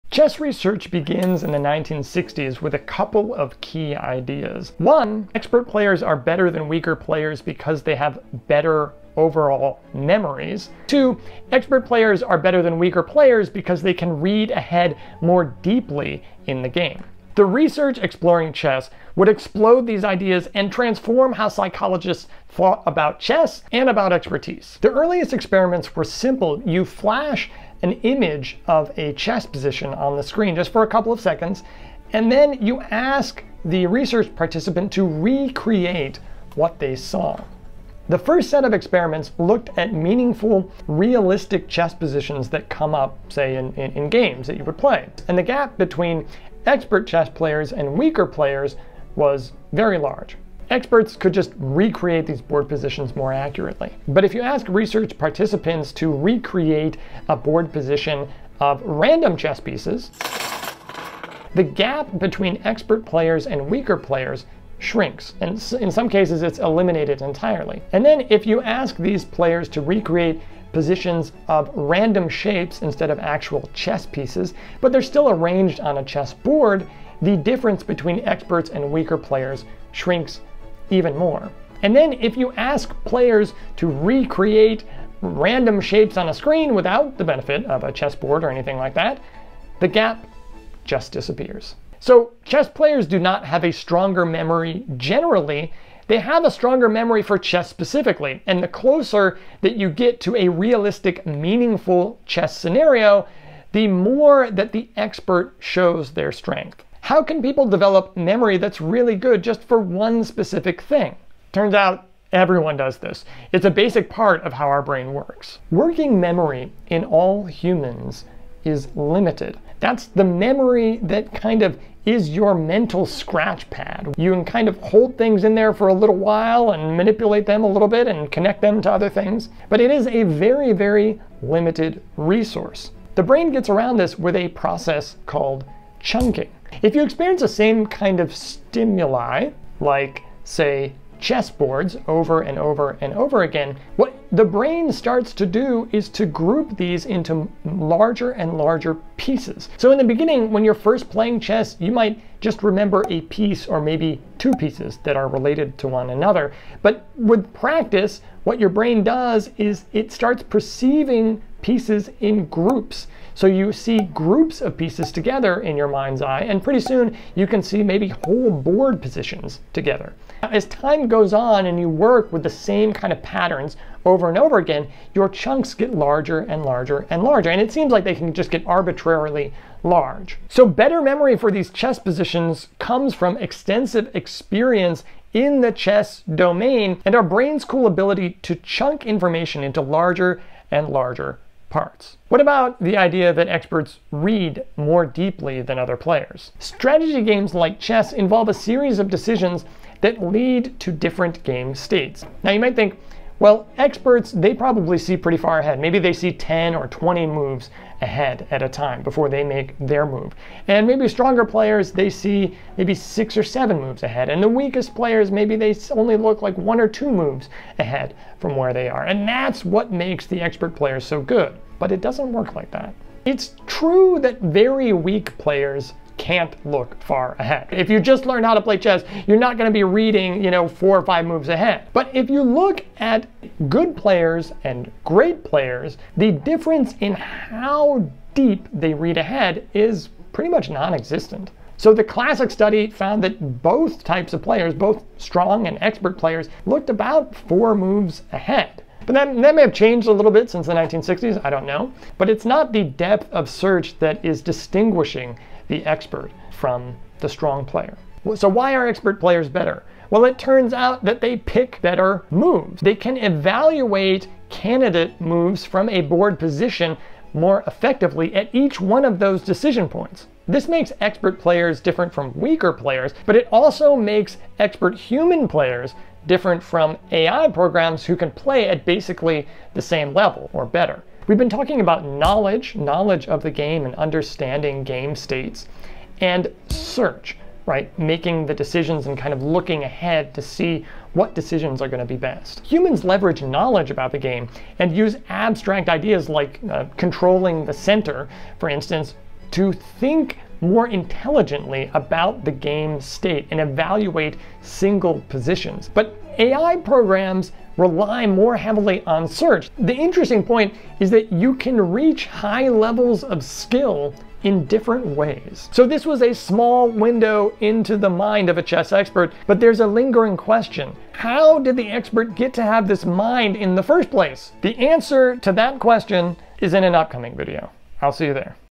Why are chess masters so much better than you?
What makes great chess players so great? What's going on in their heads? Do they just have great memories? How far do they look ahead?

00:00 Introduction
00:24 The early experiments
1:46 A brain superpower
3:12 Depth of search
4:33 Human experts vs AI chess programs

References:

My summarization of the memory results is really recounting this recent study:

Smith, E. T., Bartlett, J. C., Krawczyk, D. C., & Basak, C. (2021). Are the advantages of chess expertise on visuo-spatial working-memory capacity domain specific or domain general?. Memory & Cognition, 49(8), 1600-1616.

But there's lots of other research along the same lines.

The earliest research was here:

de Groot, A. D. (1965). Thought and choice in chess. The Hague: Mouton Publishers.

Chase, W. G., & Simon, H. A. (1973). The mind's eye in chess. In Visual information processing (pp. 215-281). Academic Press.

And here are some other solid articles I looked at:

Charness, N. (1992). The impact of chess research on cognitive science. Psychological research, 54(1), 4-9.

Bilalić, M., McLeod, P., & Gobet, F. (2008). Expert and “novice” problem solving strategies in chess: Sixty years of citing de Groot (1946). Thinking & Reasoning, 14(4), 395-408.

Connors, M. H., Burns, B. D., & Campitelli, G. (2011). Expertise in complex decision making: The role of search in chess 70 years after de Groot. Cognitive science, 35(8), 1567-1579.